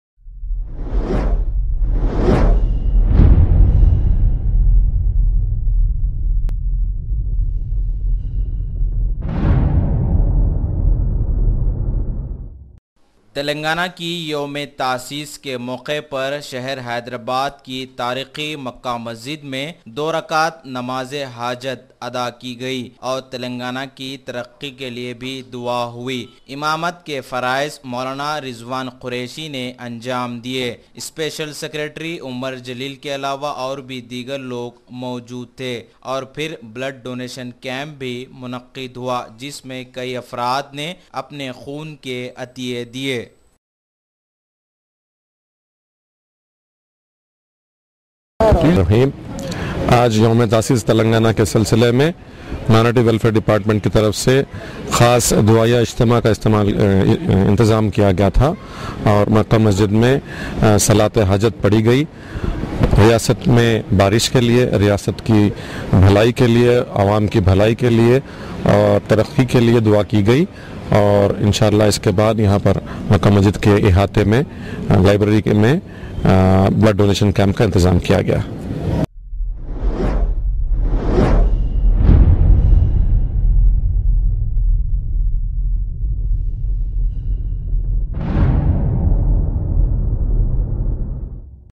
Muslims offer Salatul Hajaat Namaz and prayers for development of Telangana at Mecca Masjid
Muslims offer Salatul Hajaat Namaz and prayers for development of Telangana at Mecca Masjid and also donated blood on the occasion of 2nd Anniversary of Formation of Telangana State.